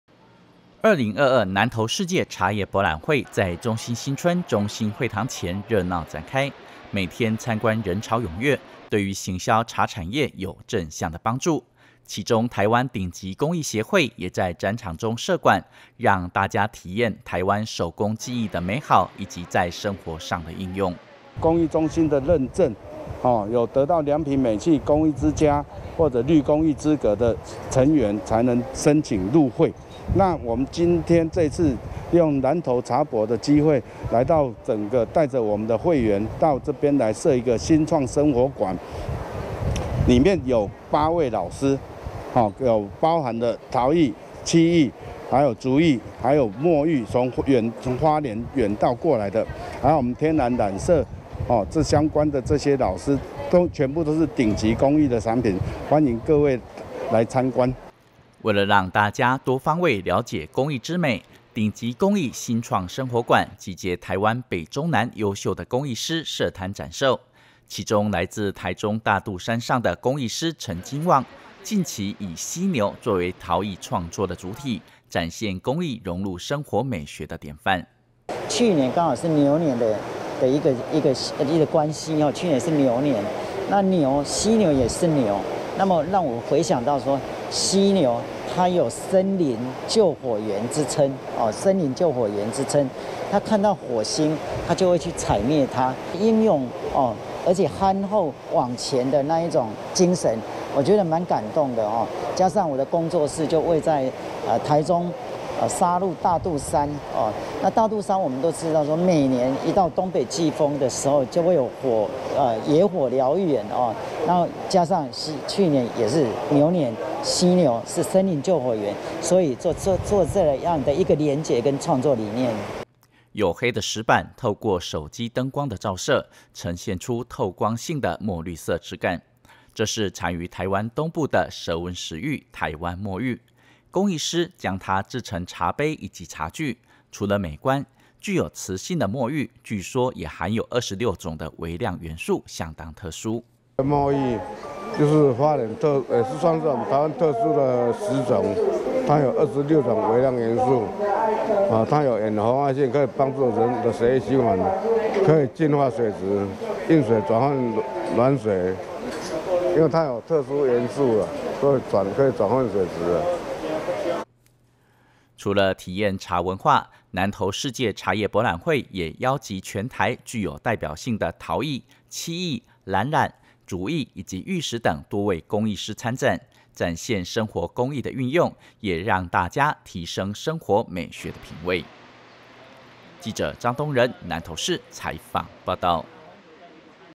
頂級工藝茶博會設展 親近新創生活展現｜南投縣政新聞 2022.10.12
百工技藝體驗生活，台灣頂級工藝協會在2022南投世界茶業博覽會(10/8~10/16)設置頂級工藝新創生活館，集結陶、漆、藍染、竹藝、玉石等多位工藝師，展售創意十足又兼具實用的生活好物。遊客也可以與工藝師們面對面交流，啟發生活美學提升工藝品味。